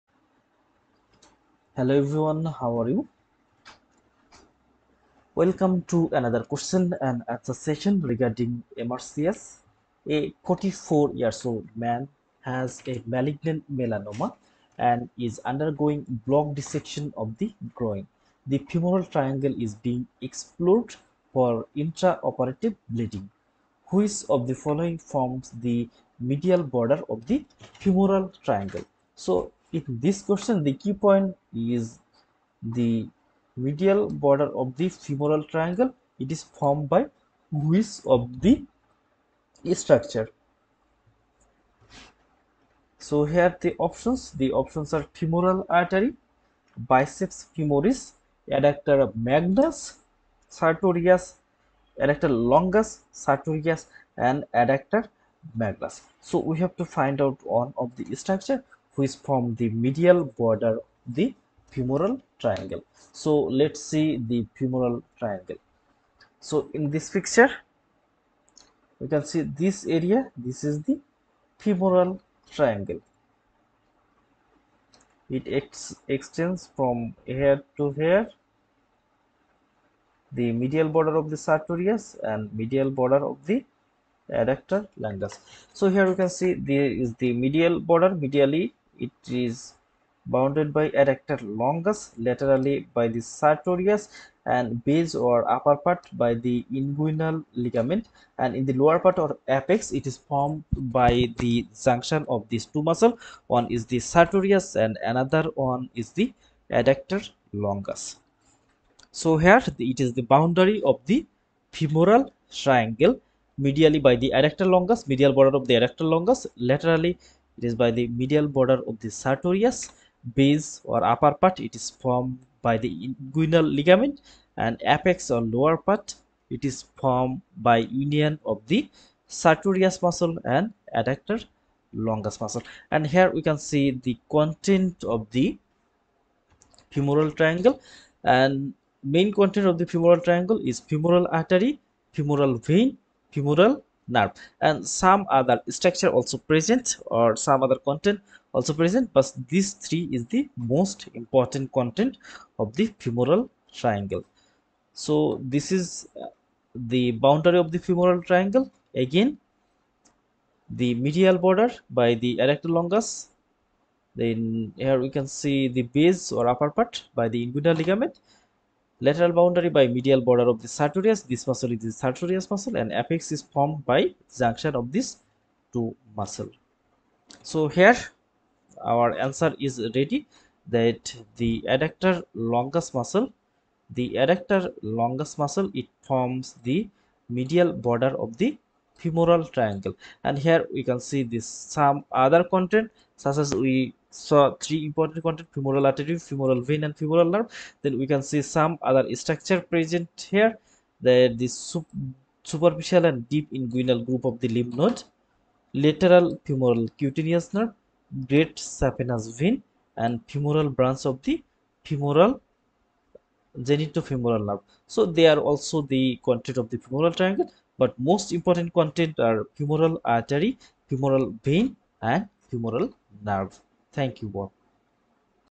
Boundary and content of femoral triangle MRCS question solve (CRACK MRCS)
it includes
1. eMRCS videos
2. Fawzia sheet solved
3. Recalls and mores